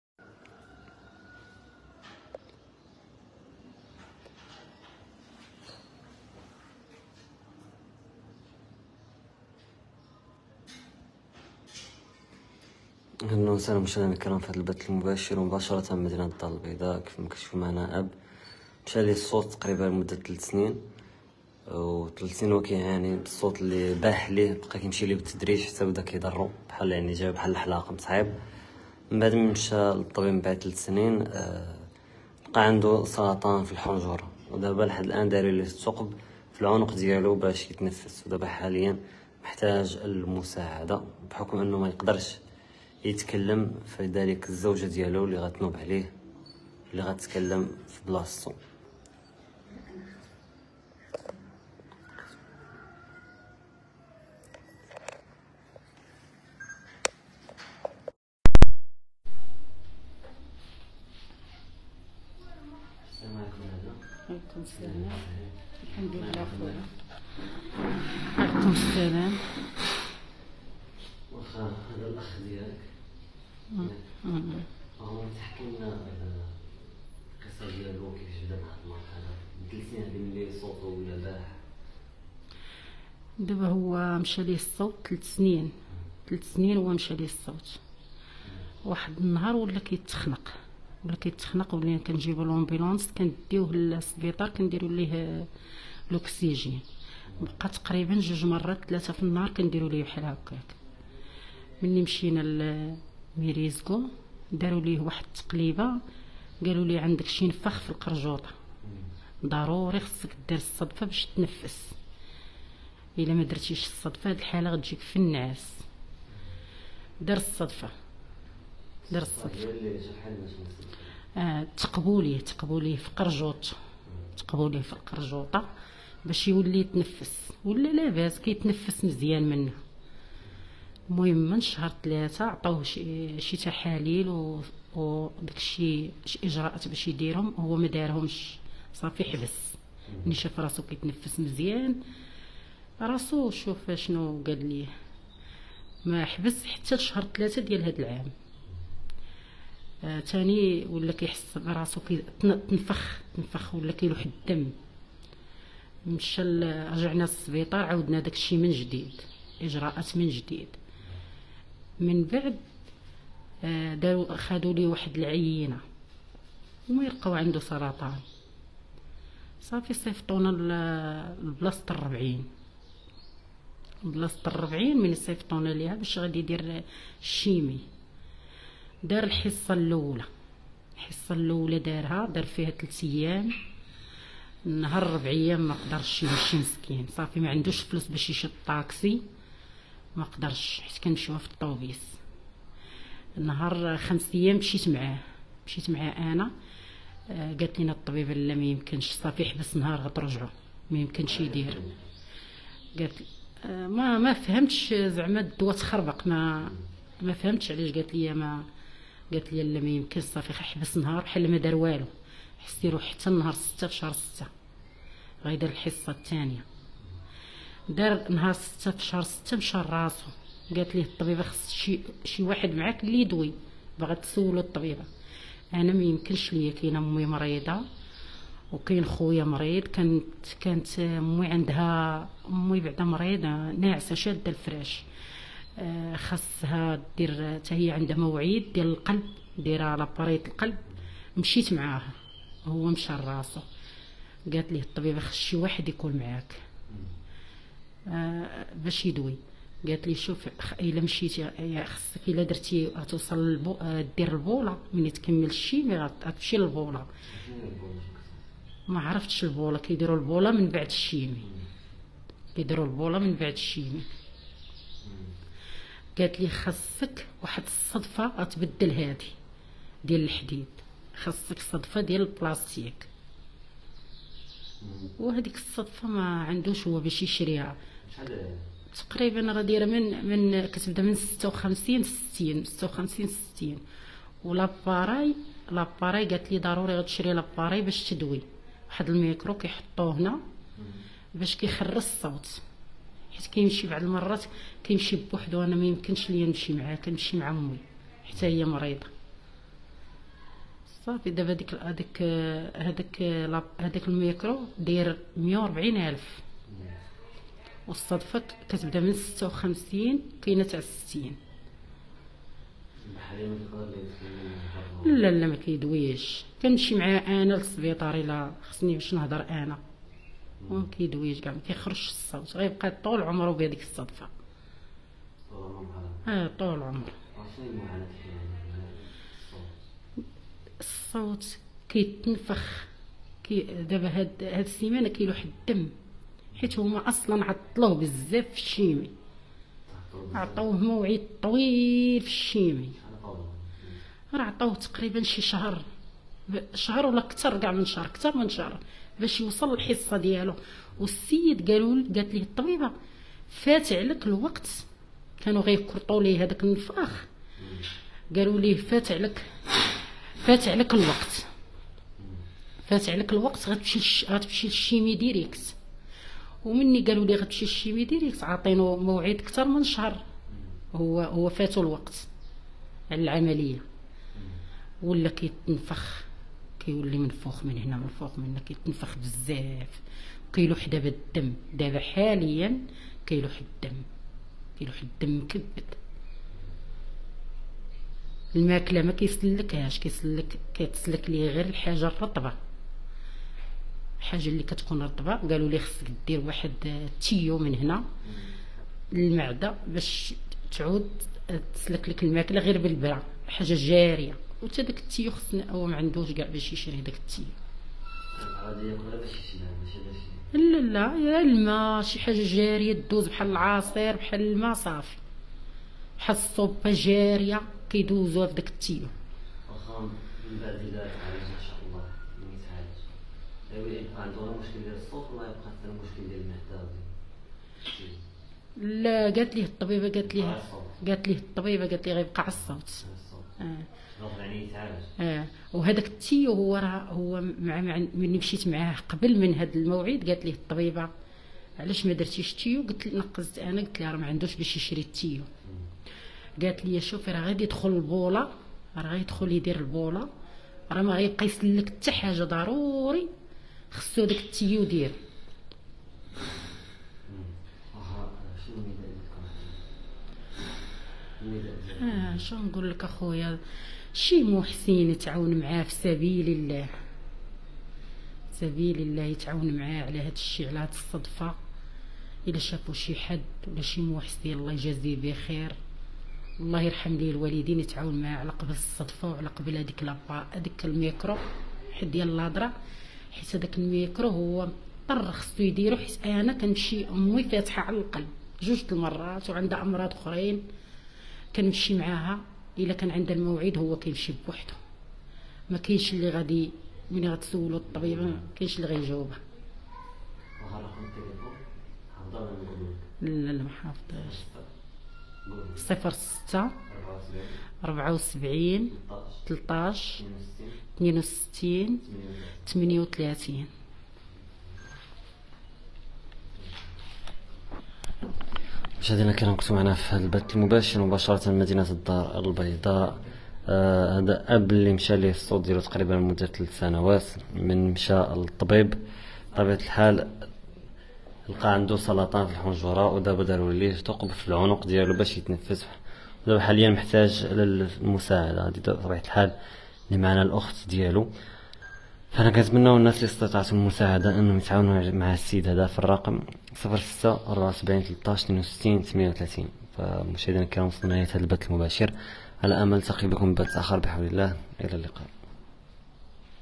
أب مشى ليه الصوت مدة 3 سنوات مين مشى لطبيب لقى عندو سرطان في الحنجرة ودبا دارو ليه ثقب في العنق
مباشرة من الدار البيضاء: أب مشى ليه الصوت مدة 3 سنوات مين مشى لطبيب لقى عندو سرطان في الحنجرة ودبا دارو ليه ثقب في العنق باش يتنفس و محتاج للمساعدة.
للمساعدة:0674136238